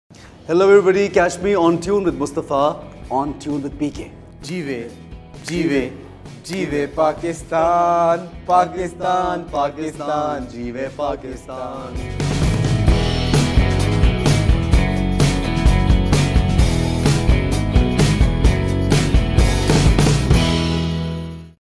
Interview with Ahsan Khan (Teaser) Episode 14
Host: Mustafa Shah
Production: 1mm Arts
Grooming: Blazon Salon and Studio
Location: Patli Gali, MM Alam Road, Lahore
Release Date: 19 December, 2016